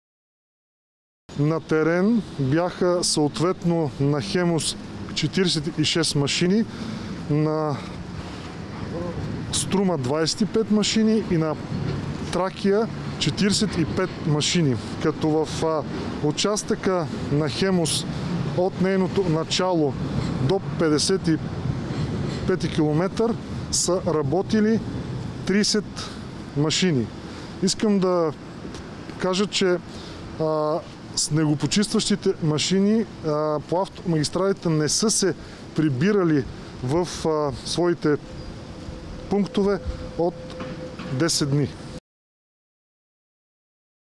От 10 дни не са се прибирали снегопочистващите машини
Троянският проход остава затворен заради снежната покривка. Обилен сняг вали и в Родопите, в областите Смолян и Кърджали има поетапни ограничения за тежкотоварните автомобили. Това съобщи председателят на Агенция „Пътна инфраструктура“ Георги Терзийски.